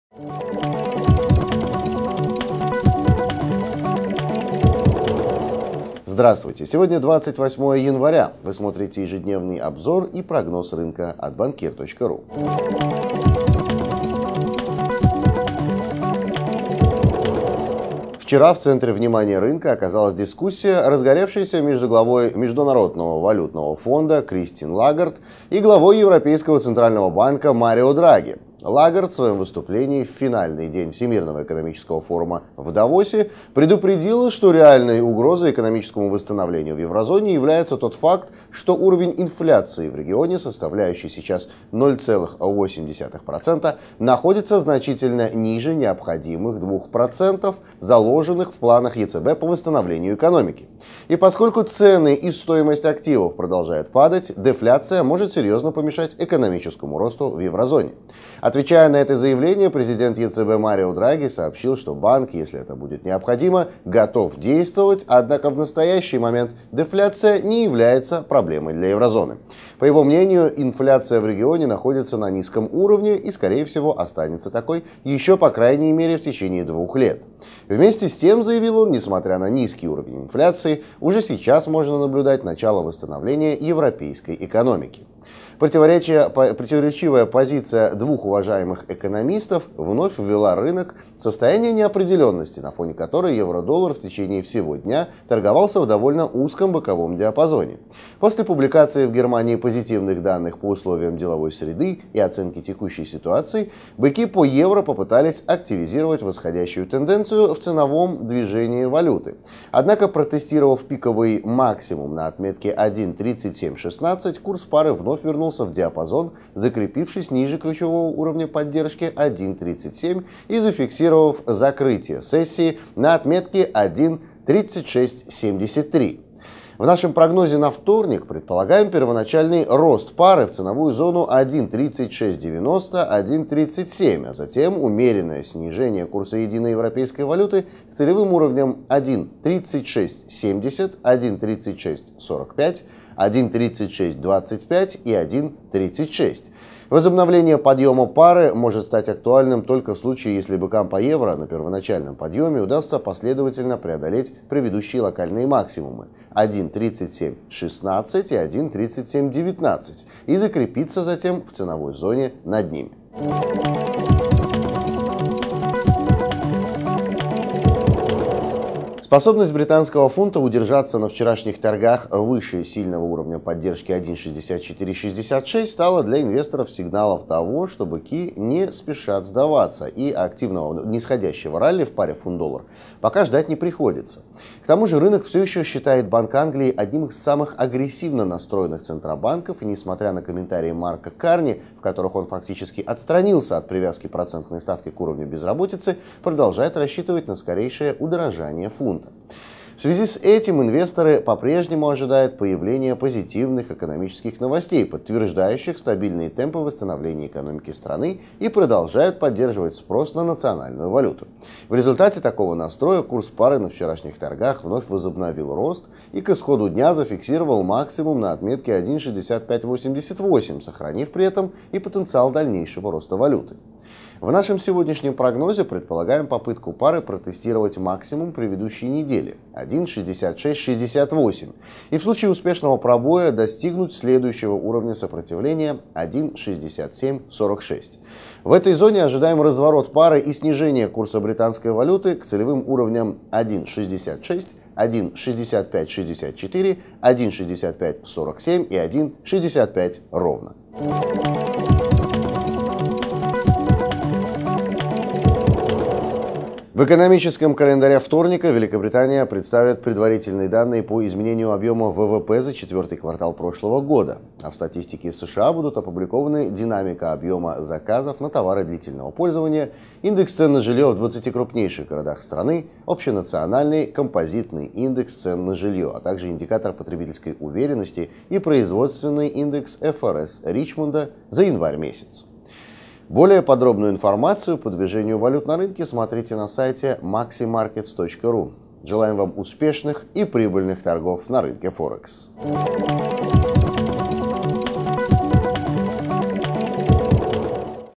Daily-28.01.2014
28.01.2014. Bankir-TV-MaxiForex. Бонус до 5000$

Дневной видео обзор мирового валютного рынка Форекс (FOREX) от и Финансовые новости, прогноз курсов валют - евро/доллар, фунт/доллар Читайте